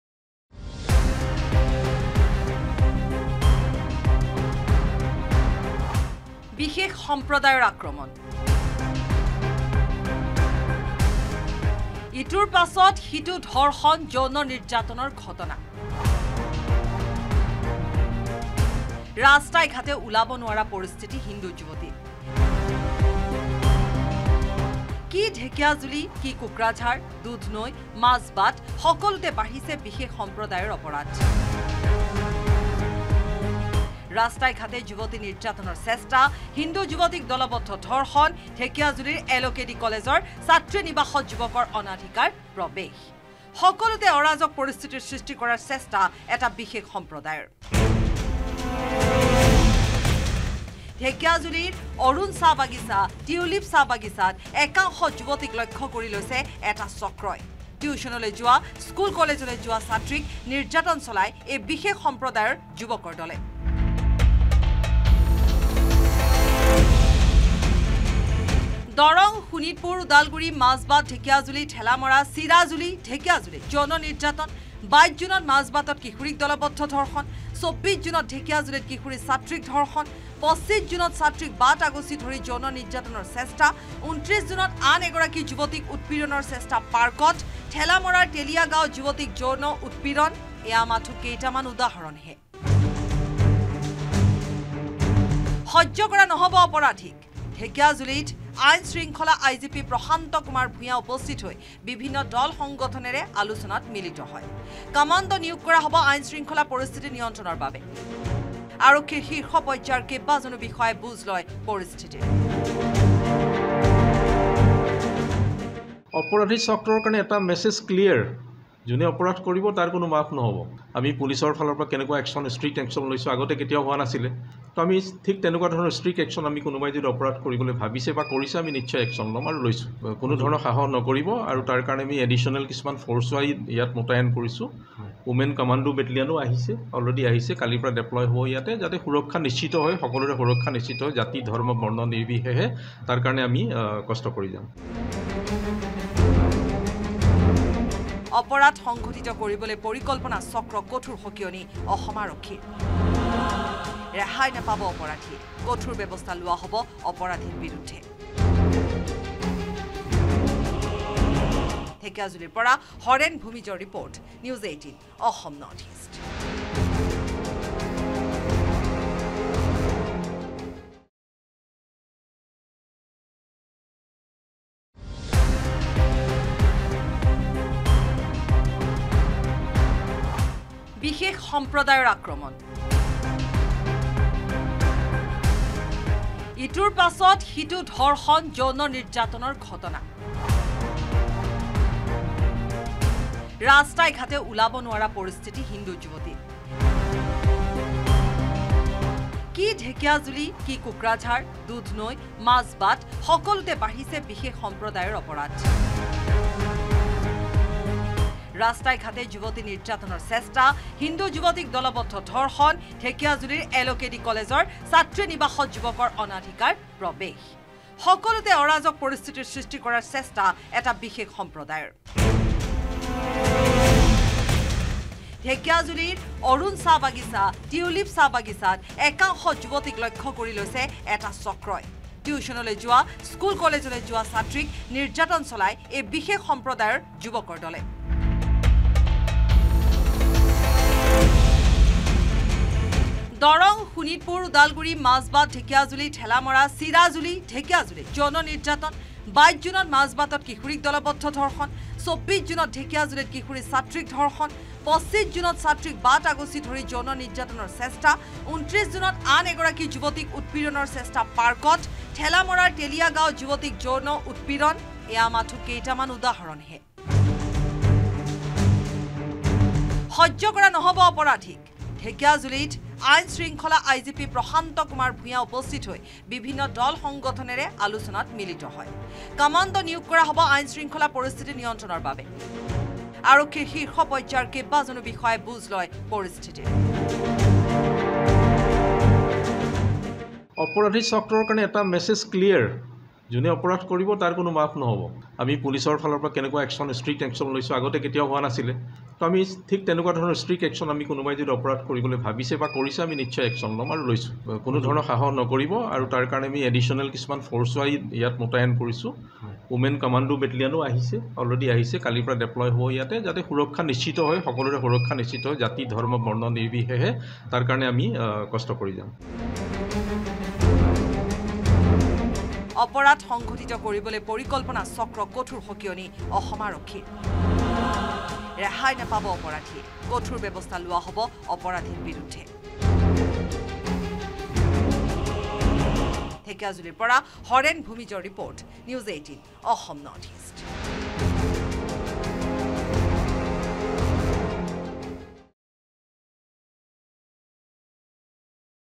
Assam News : ৰাজ্যত ধাৰাবাহিক ধৰ্ষণ-যৌন নিৰ্যাতনৰ ঘটনা | N18V
Assam News | এটা বিশেষ সম্প্ৰদায়ৰ টাৰ্গেট হৈ পৰিছে হিন্দু যুৱতী-কিশোৰী। ৰাজ্যত সংঘটিত ইটোৰ পিছত সিটো ধৰ্ষণ তথা যৌন নিৰ্যাতনৰ ঘটনাই উদ্বিগ্ন কৰি তুলিছে ৰাজ্যবাসীক। ইপিনে ধাৰাবাহিক জঘন্য ঘটনাৰ পিছতে কঠোৰ হৈ পৰিছে অসম আৰক্ষী।